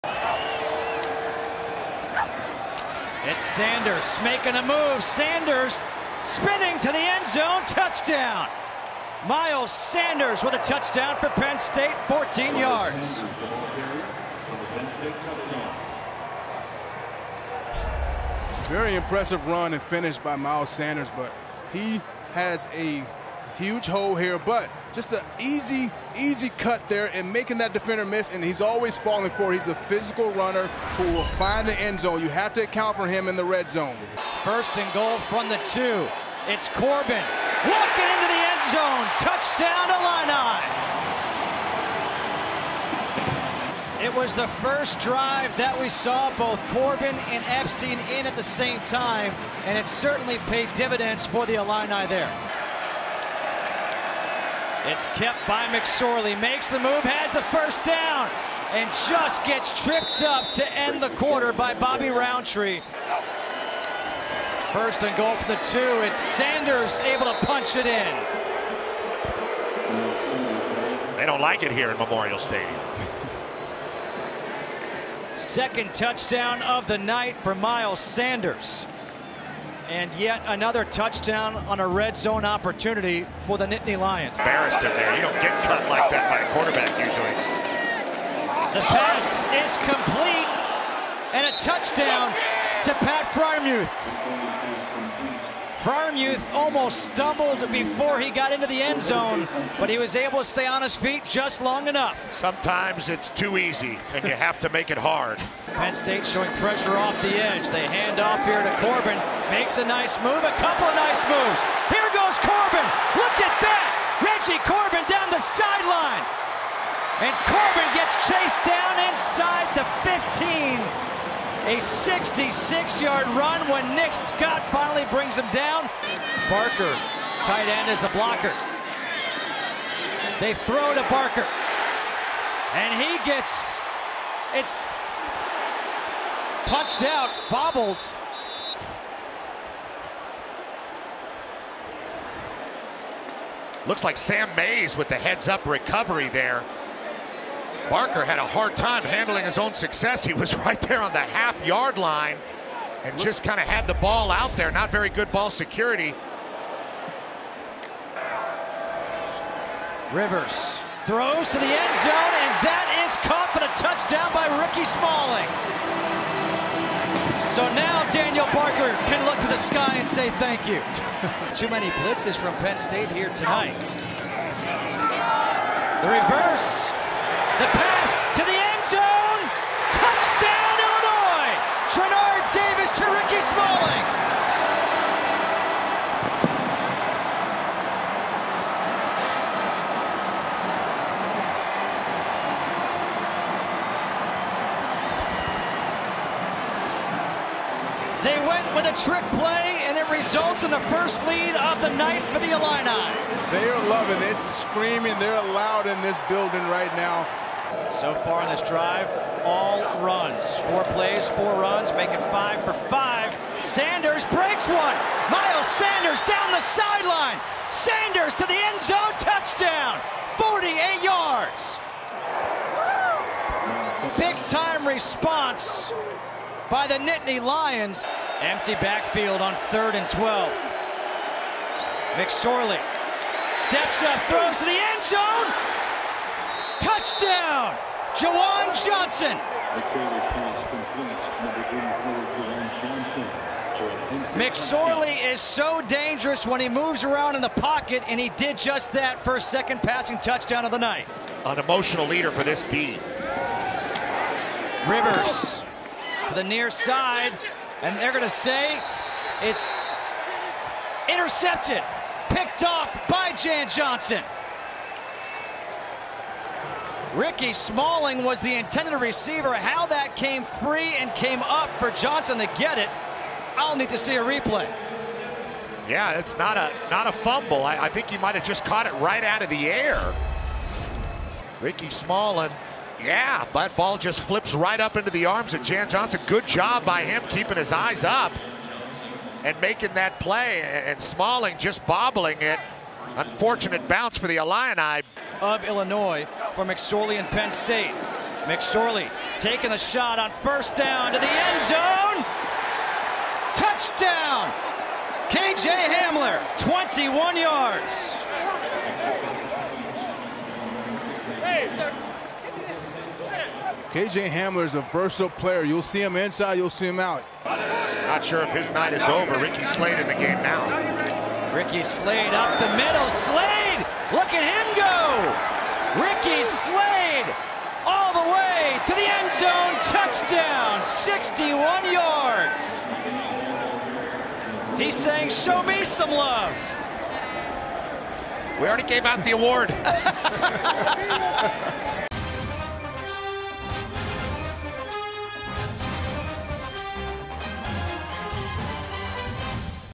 Penn State vs. Illinois | FOX COLLEGE FOOTBALL HIGHLIGHTS
Penn State drops 35 points in the 4th quarter to crush Illinois 63-24.